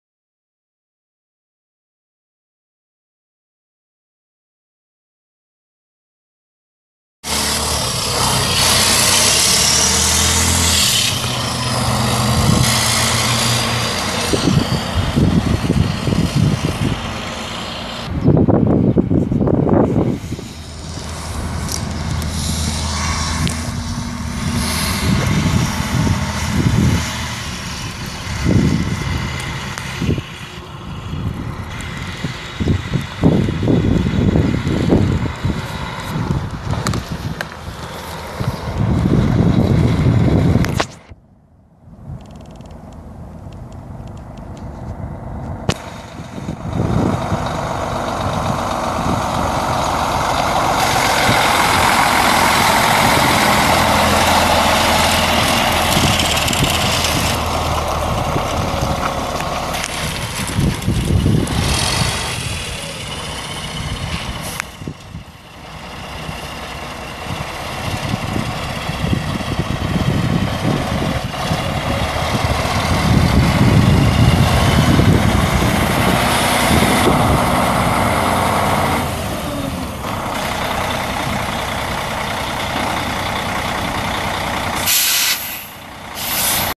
2005 Freightliner Columbia semi truck for sale | no-reserve Internet auction April 6, 2017
2005 Freightliner Columbia semi truck
1,032,312 miles on odometer
22,671 hours on meter
Mercedes-Benz 0M460LA six cylinder turbo diesel engine
Smart shift automatic transmission
Tandem axle
Tilt
Telescoping steering wheel
Cruise
AM/FM/CD
50" sleeper
68"H I.D.
Single bed
Storage cabinets
Rear climate control
Hanging closet
Cab extenders
Chrome bumper
Dump valves
Exterior visor
Mudflaps
Air suspension
Air brakes
Engine brake
228" wheelbase
Aluminum wheels
275/80R24.5 front tires
455/55R22.5 rear tires
Unit# 6080
Nebraska title
1FUJA6CV75DN87849
It's located in Plattsmouth, NE, and we're selling it to the highest bidder regardless of price.